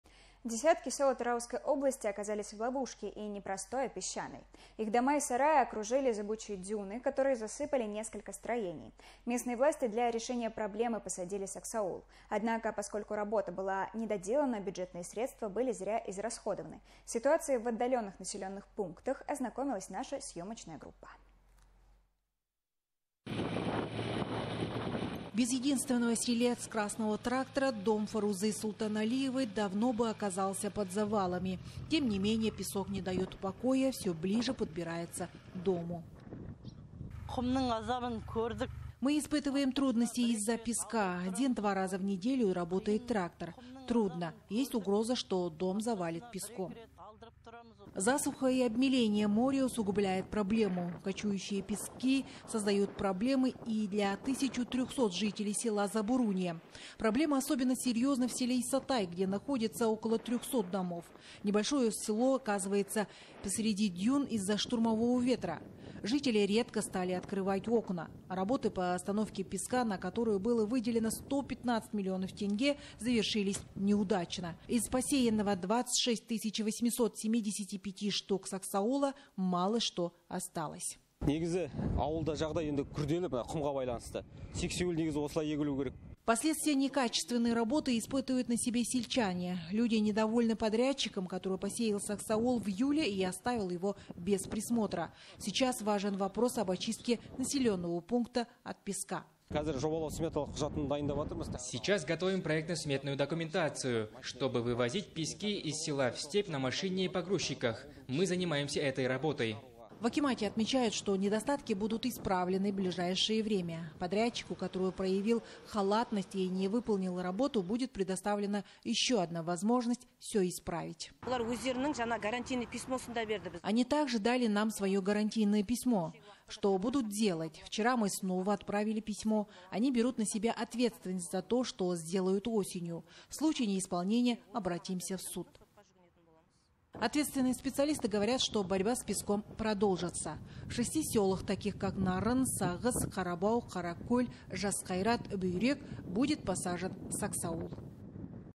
ДЕСЯТКИ СЕЛ ОКАЗАЛИСЬ В ПЕСЧАНОЙ ЛОВУШКЕ
Десятки сел Атырауской области оказались в ловушке, и не простой, а песчаной. Их дома и сараи окружили зыбучие дюны, которые засыпали несколько строений. Местные власти для решения проблемы посадили саксаул. Однако, поскольку работа была недоделана, бюджетные средства были зря израсходованы. С ситуацией в отдаленных населенных пунктах ознакомилась наша сьемочная группа.